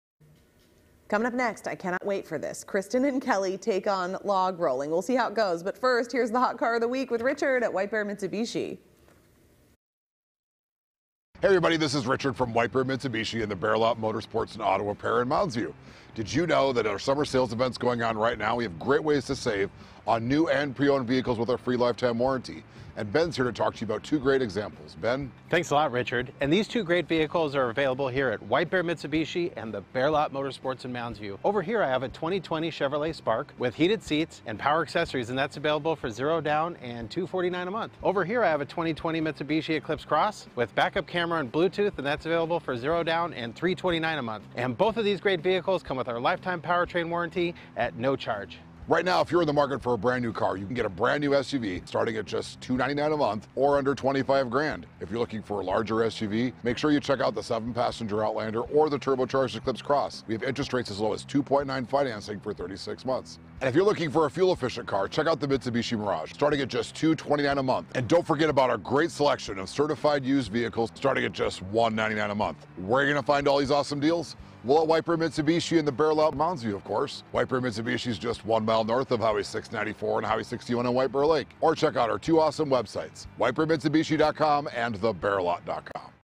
White Bear Mitsubishi Hot Car of the Week - Aug 22
Right now, it's our Summer Sales Event, and we've got great deals on new and certified pre-owned vehicles with our free lifetime warranty!

2020 Chevrolet Spark
🔥Buy from $15,996, OR
🔥Finance for $249/month with $0 Down

2020 Mitsubishi Eclipse Cross
🔥Buy from $21,596, OR
🔥Finance for $329/month with $0 Down

If you're in the market for a brand new car, you can get a new SUV starting at just $299/month or under $25,000. If you're looking for a larger SUV make sure you check out the seven passenger Outlander or the turbocharged Eclipse Cross. We have interest rates as low as 2.9 financing for 36 months. If you are looking for a fuel efficient car, check out the Mitsubishi Mirage starting at just $229/month. Don't forget about our great selection of certified used vehicles starting at just $199/month.